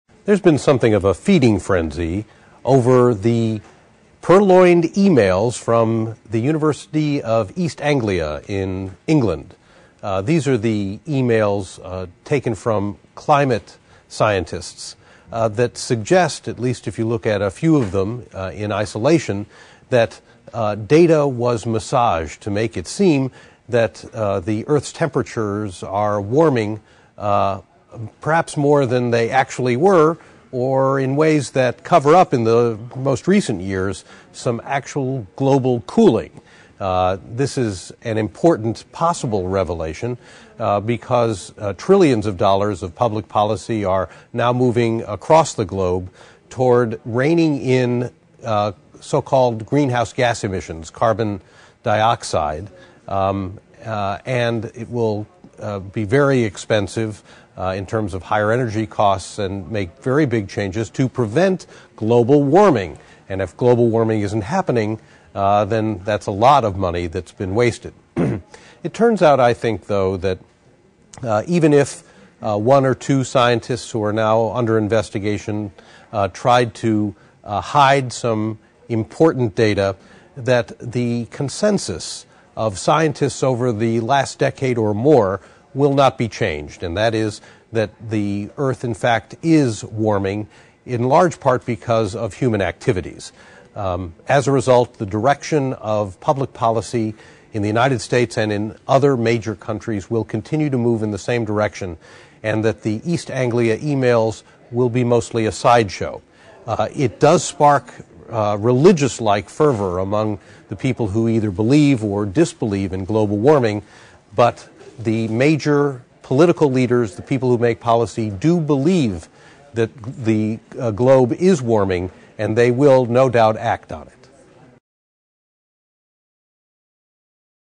Birnbaum on Washington (Dec. 3, 2009)
Jeff Birnbaum discusses the stolen emails from British and US scientists from the University of East Anglia late last month and what they mean politically.